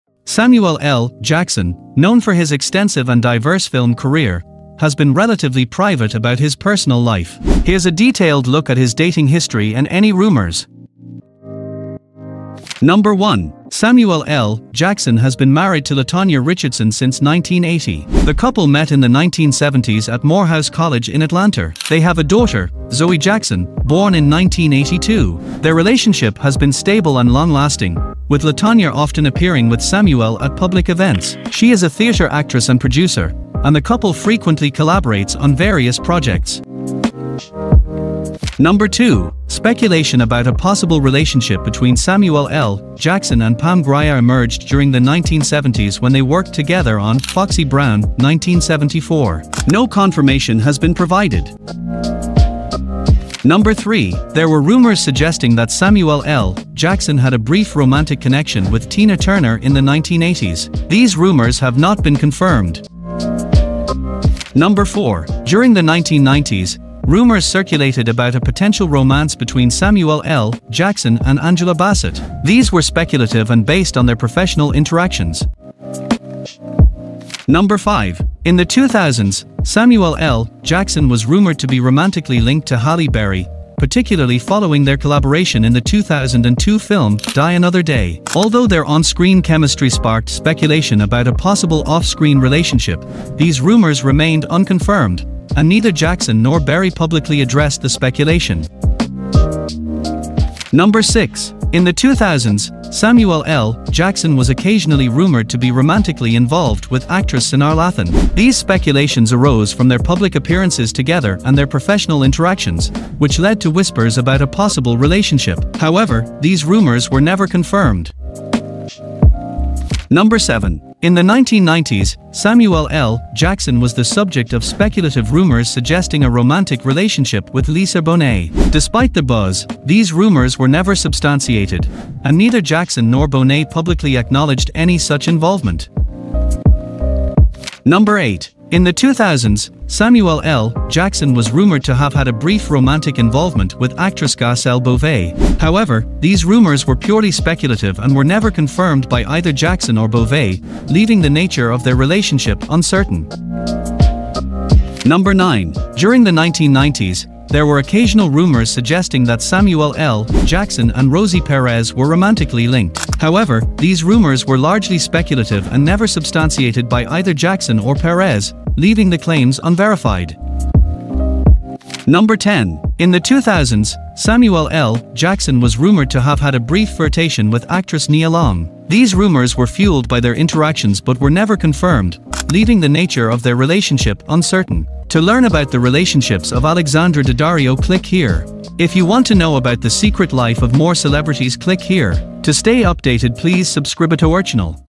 Samuel L. Jackson - All Girlfriends (1974 - Present)
Samuel L. Jackson, known for his extensive and diverse film career, has been relatively private about his personal life. Here’s a detailed look at his dating history and any rumors.
Number 1. Samuel L. Jackson has been married to LaTanya Richardson since 1980. The couple met in the 1970s at Morehouse College in Atlanta. They have a daughter, Zoe Jackson, born in 1982. Their relationship has been stable and long-lasting, with LaTanya often appearing with Samuel at public events. She is a theater actress and producer, and the couple frequently collaborates on various projects.
Number 2. Speculation about a possible relationship between Samuel L. Jackson and Pam Grier emerged during the 1970s when they worked together on "Foxy Brown" (1974). No confirmation has been provided.
Number 3. There were rumors suggesting that Samuel L. Jackson had a brief romantic connection with Tina Turner in the 1980s. These rumors have not been confirmed.
Number 4. During the 1990s, rumors circulated about a potential romance between Samuel L. Jackson and Angela Bassett. These were speculative and based on their professional interactions.
Number 5. In the 2000s, Samuel L. Jackson was rumored to be romantically linked to Halle Berry, particularly following their collaboration in the 2002 film "Die Another Day." Although their on-screen chemistry sparked speculation about a possible off-screen relationship, these rumors remained unconfirmed, and neither Jackson nor Berry publicly addressed the speculation.
Number 6. In the 2000s, Samuel L. Jackson was occasionally rumored to be romantically involved with actress Sanaa Lathan. These speculations arose from their public appearances together and their professional interactions, which led to whispers about a possible relationship. However, these rumors were never confirmed
Number 7. In the 1990s, Samuel L. Jackson was the subject of speculative rumors suggesting a romantic relationship with Lisa Bonet. Despite the buzz, these rumors were never substantiated, and neither Jackson nor Bonet publicly acknowledged any such involvement
Number 8. In the 2000s, Samuel L. Jackson was rumored to have had a brief romantic involvement with actress Garcelle Beauvais. However, these rumors were purely speculative and were never confirmed by either Jackson or Beauvais, leaving the nature of their relationship uncertain.
Number 9. During the 1990s, there were occasional rumors suggesting that Samuel L. Jackson and Rosie Perez were romantically linked. However, these rumors were largely speculative and never substantiated by either Jackson or Perez, leaving the claims unverified.
Number 10. In the 2000s, Samuel L. Jackson was rumored to have had a brief flirtation with actress Nia Long. These rumors were fueled by their interactions but were never confirmed, leaving the nature of their relationship uncertain.